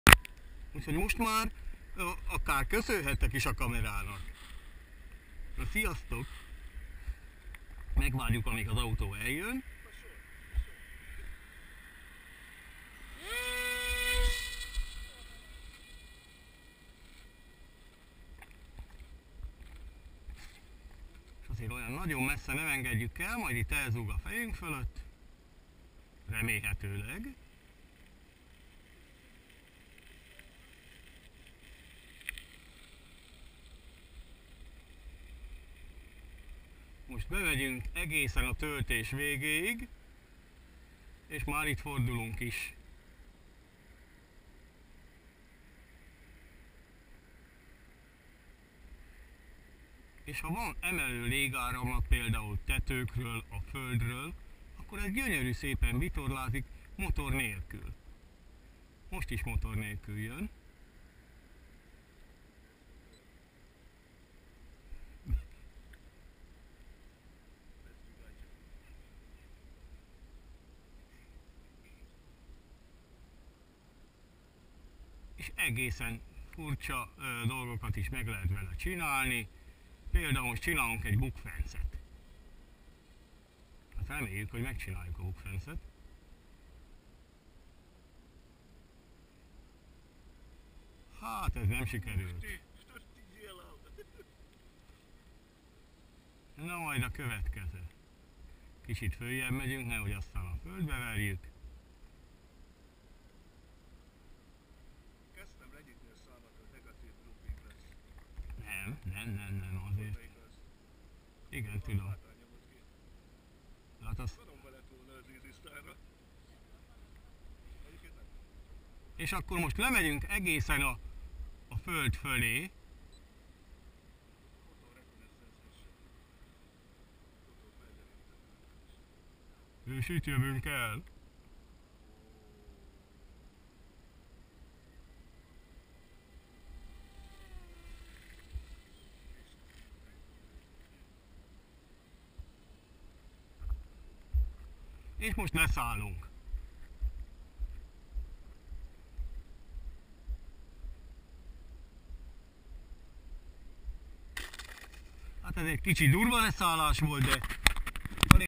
Virág, Flóra Jázmin és Kornél látogatása a Helios UNU-n. (Répcelak)
Virág, Kornél és az elbűvölő Flóra Jázmin megtekintette a Helios UNU-n (Répce I.) az éjszakai repülés előkészületeit és az első 3 leszállást is. Érdeklődve figyelték, ahogy az Easy Star hasította a levegőeget a magsban, vagy éppen földközelben 1-2 m magasságban kúszott át a HELIOS UNU fölött. Reméljük, hogy többször is tiszteletüket teszik!